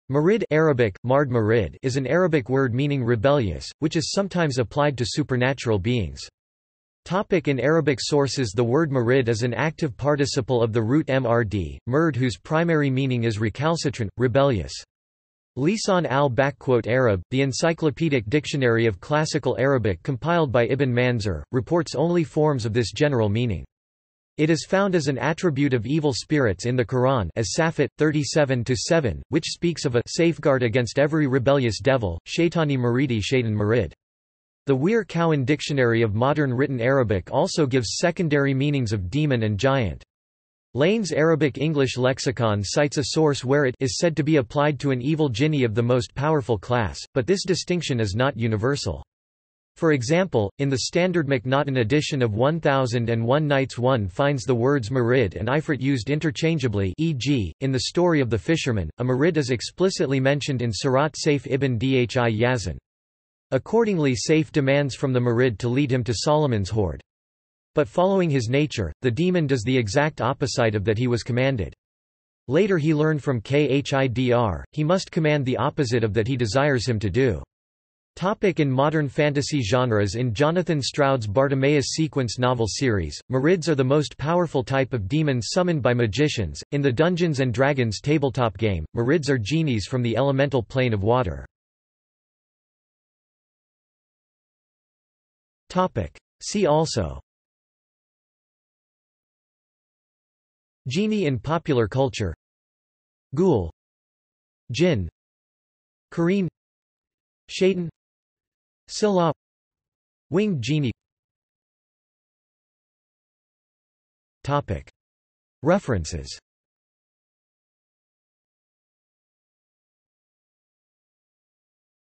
Marid | Wikipedia audio article
This is an audio version of the Wikipedia Article:
Marid

- Socrates

SUMMARY
Marid (Arabic: مارد‎ mārid) is an Arabic word meaning rebellious, which is sometimes applied to supernatural beings.